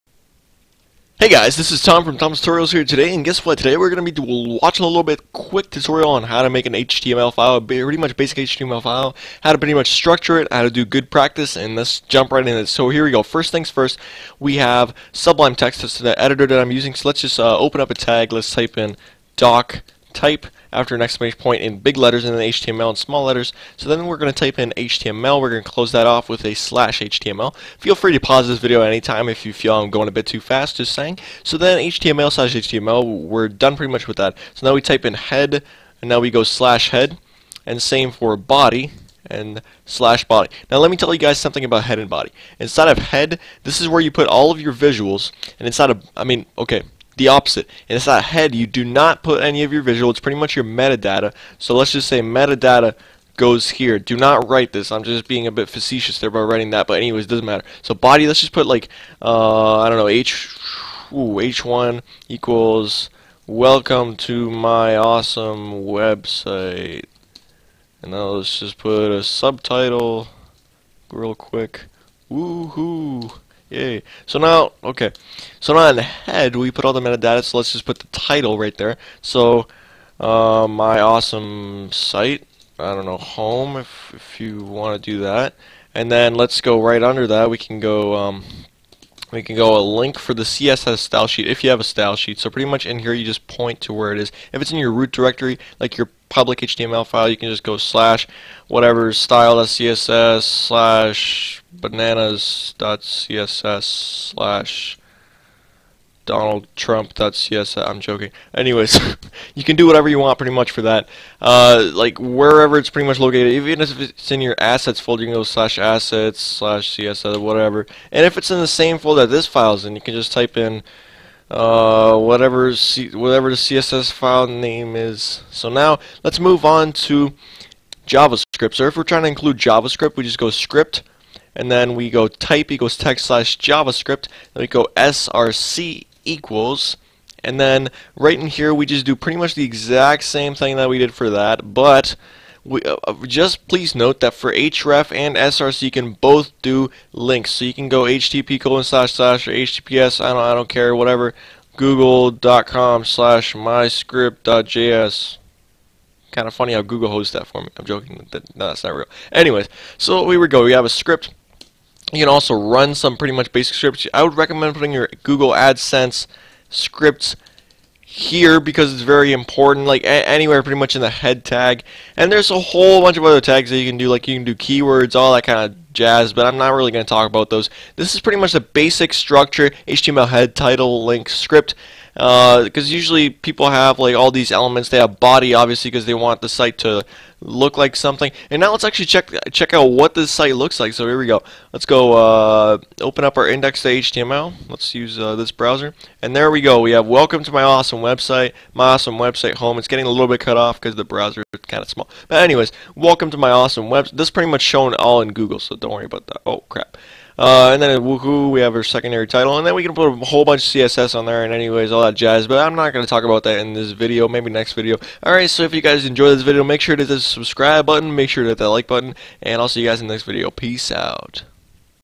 How to Make a Basic HTML File - Best Practice!
I show you how to make a basic HTML file including the basic code markup.